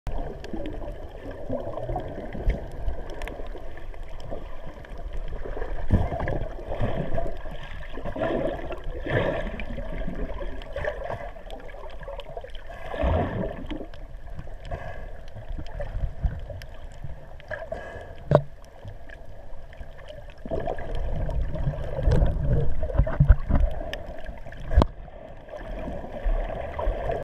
Big ass eel
Here is a big ass eel I saw snorkeling in cuba. shot with my gopro